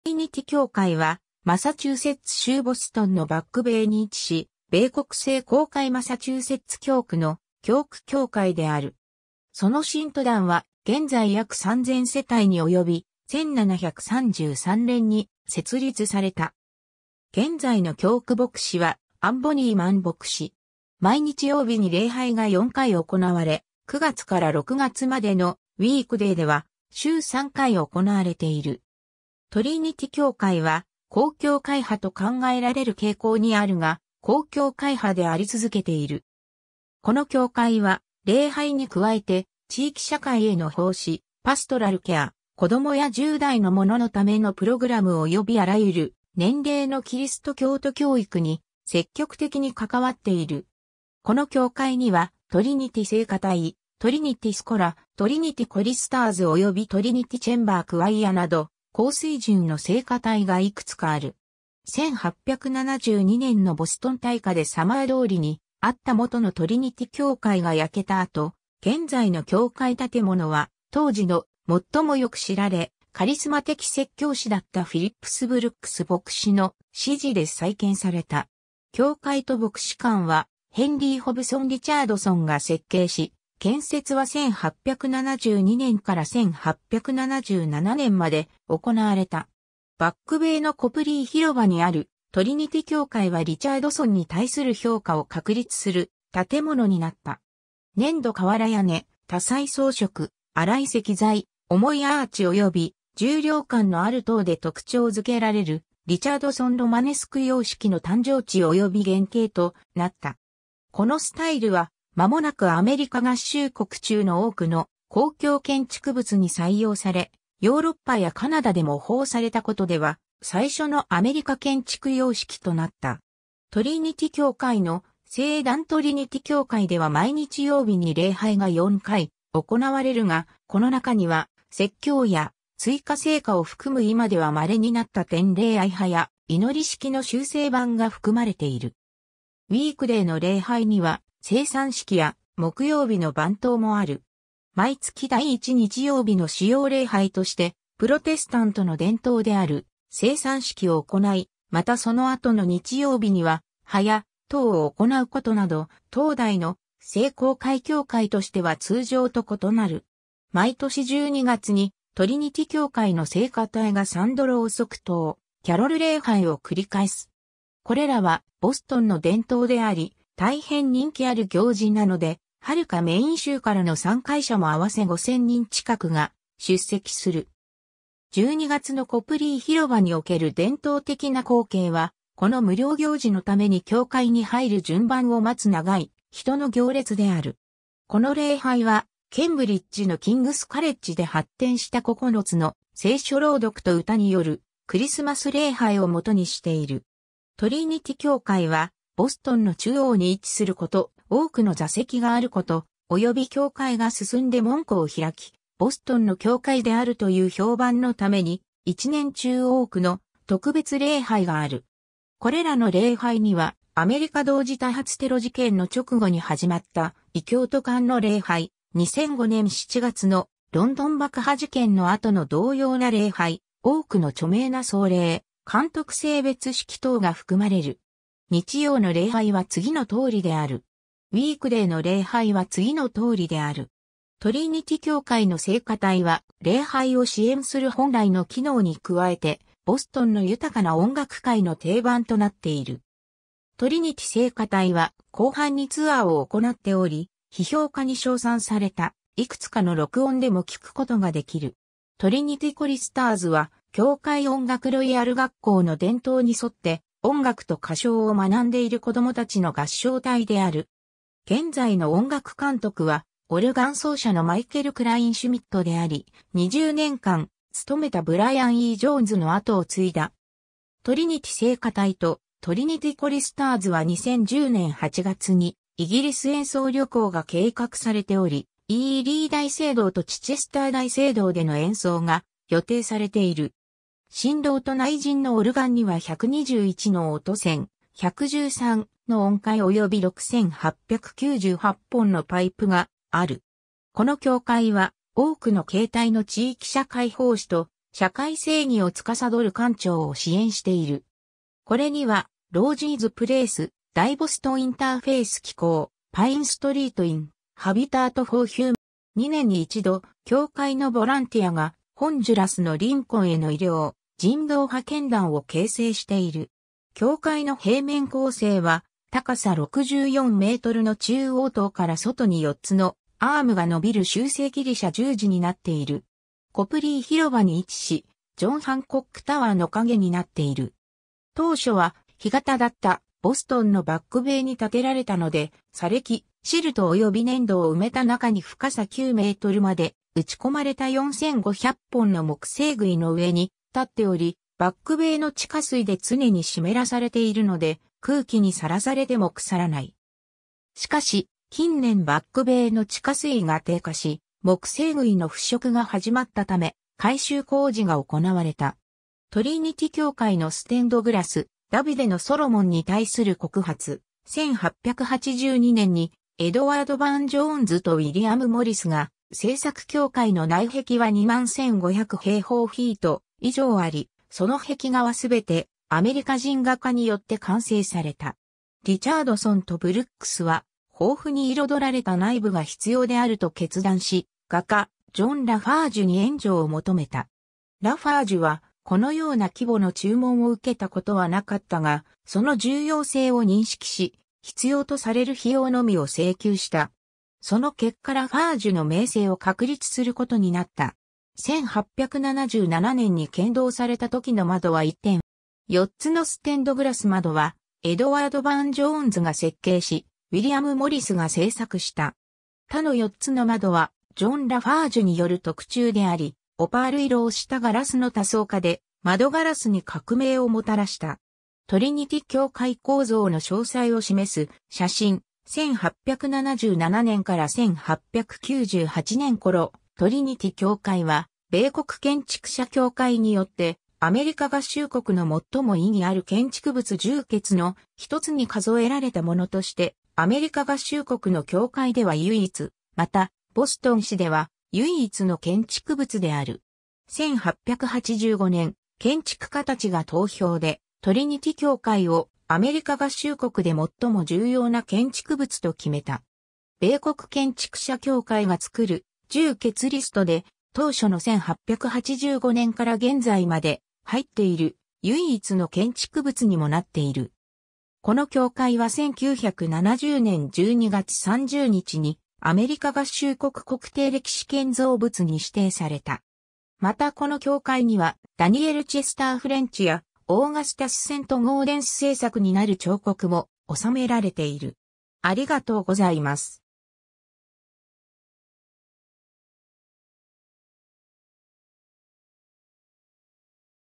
トリニティ教会 (ボストン)
トリニティ教会（トリニティきょうかい、英: Trinity Church）は、マサチューセッツ州ボストンのバックベイに位置し、米国聖公会マサチューセッツ教区の教区教会である。
その信徒団は現在約3,000世帯に及び、1733年に設立された。
現在の教区牧師はアン・ボニーマン牧師。
毎日曜日に礼拝が4回行われ、9月から6月までのウィークデーでは週3回行われている。
トリニティ教会は「高教会派」と考えられる傾向にあるが、広教会派であり続けている。
 この教会は礼拝に加えて、地域社会への奉仕、パストラルケア（患者とその家族の心のケア）、子供や10代の者のためのプログラムおよびあらゆる年齢のキリスト教徒教育に積極的に関わっている。
 この教会にはトリニティ聖歌隊、トリニティ・スコラ、トリニティ・コリスターズおよびトリニティ・チェンバー・クワイアなど高水準の聖歌隊が幾つかある。
 1872年のボストン大火でサマー通りにあった元のトリニティ教会が焼けた後、現在の教会建物は当時の最も良く知られカリスマ的説教師だったフィリップス・ブルックス牧師（1835年-1893年）の指示で再建された。
教会と牧師館はヘンリー・ホブソン・リチャードソンが設計し、建設は1872年から1877年まで行われた。
バックベイのコプリー広場にあるトリニティ教会はリチャードソンに対する評価を確立する建物になった。
粘土瓦屋根、多彩装飾、粗い石材、重いアーチおよび重量感のある塔で特徴付けられるリチャードソン・ロマネスク様式の誕生地および原形となった。
このスタイルは間もなくアメリカ合衆国中の多くの公共建築物に採用され、ヨーロッパやカナダで模倣されたことでは最初のアメリカ建築様式となった。
 トリニティ教会の聖壇 トリニティ教会では毎日曜日に礼拝が4回行われるが、この中には説教や追加聖歌を含む今では希になった典礼I早祷式の修正版が含まれている。
ウィークデーの礼拝には聖餐式や木曜日の晩祷もある。
毎月第1日曜日の主要礼拝としてプロテスタントの伝統である正餐式を行い、またその後の日曜日には早祷を行うことなど、当代の聖公会教会としては通常と異なる。
 毎年12月にトリニティ教会の聖歌隊が3度蝋燭灯キャロル礼拝を繰り返す。
これらは「ボストンの伝統」であり、大変人気有る行事なので、遙かメイン州からの参会者も合わせ5,000人近くが出席する。
12月のコプリー広場における伝統的な光景は、この無料行事のために教会に入る順番を待つ長い人の行列である。
この礼拝はケンブリッジのキングス・カレッジで発展した9つの聖書朗読と歌によるクリスマス礼拝を基にしている。
 トリニティ教会は、ボストンの中央に位置すること、多くの座席があること、および教会が進んで門戸を開き、「ボストンの教会」であるという評判のために、一年中多くの特別礼拝がある。
これらの礼拝には、アメリカ同時多発テロ事件の直後に始まった異教徒間（キリスト教徒、ユダヤ教徒およびイスラム教徒）の礼拝、2005年7月のロンドン爆破事件の後の同様な礼拝、多くの著名な葬礼、監督聖別式等が含まれる。
 日曜の礼拝は次の通りである（注釈の無い限り全て典礼II）。
 ウィークデーの礼拝は次の通りである。
 トリニティ教会の聖歌隊は礼拝を支援する本来の機能に加えて、ボストンの豊かな音楽界の定番となっている。
トリニティ聖歌隊は広範にツアーを行っており、批評家に称賛された幾つかの録音でも聞くことができる。
トリニティ・コリスターズは教会音楽ロイヤル学校の伝統に沿って音楽と歌唱を学んでいる子供達の合唱隊である。
現在の音楽監督はオルガン奏者のマイケル・クラインシュミットであり、20年間務めたブライアン・E・ジョーンズの後を継いだ。
トリニティ聖歌隊とトリニティ・コリスターズは2010年8月にイギリス演奏旅行が計画されており、イーリー大聖堂とチチェスター大聖堂での演奏が予定されている。
 身廊と内陣のオルガンには121の音栓、113の音階および6,898本のパイプがある。
 この教会は多くの形態の地域社会奉仕と社会正義を司る官庁を支援している。
これにはロージーズ・プレース、大ボストン・インターフェース機構、パイン・ストリート・イン、ハビタート・フォー・ヒューマニティ、コミュニティ・サービングス、ウォーク・フォーハンガー、ロードマンリド・フォーキッズなどとの共同事業が含まれている。